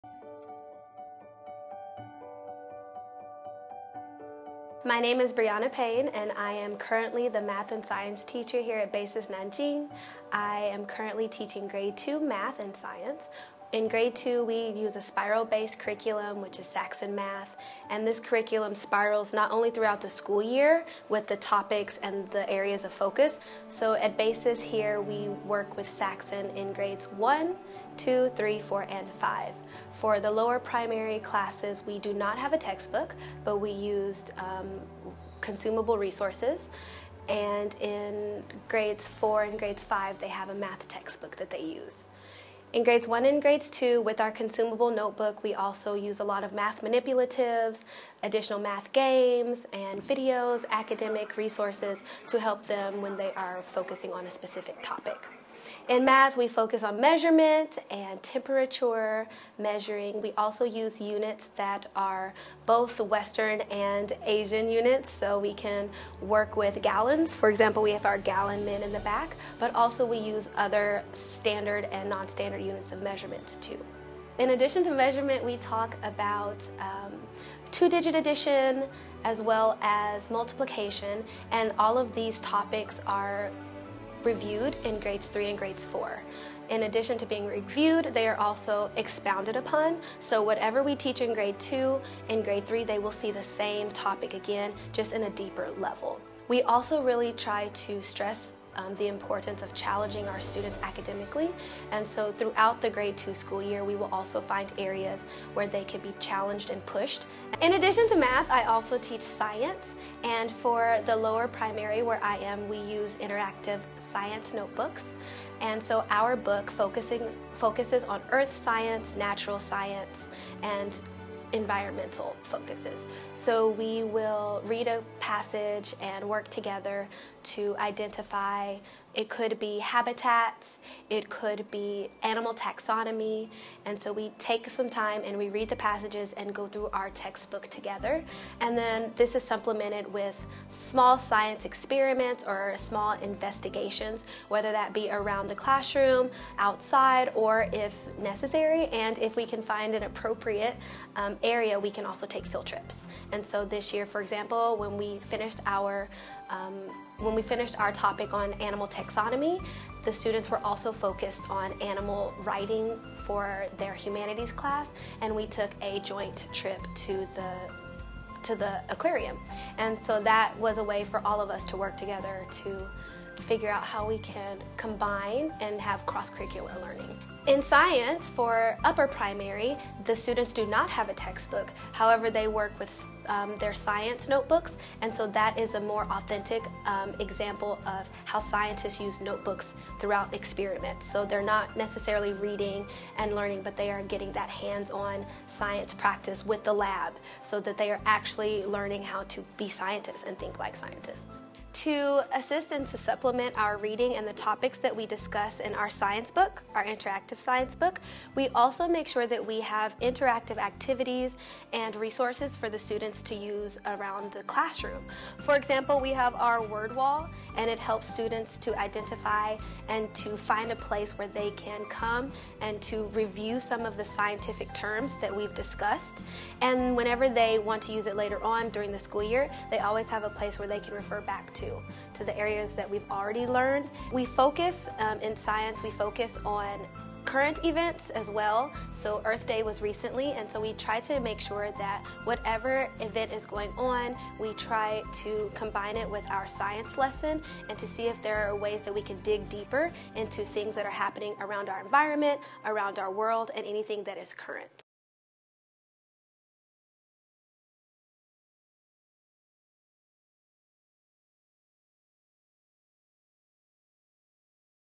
Primary Math and Science Teaching at BASIS International Schools
Hear from one of our Primary Math & Science teachers at BASIS International School Nanjing about the curriculum and benefits of teaching humanities at our schools.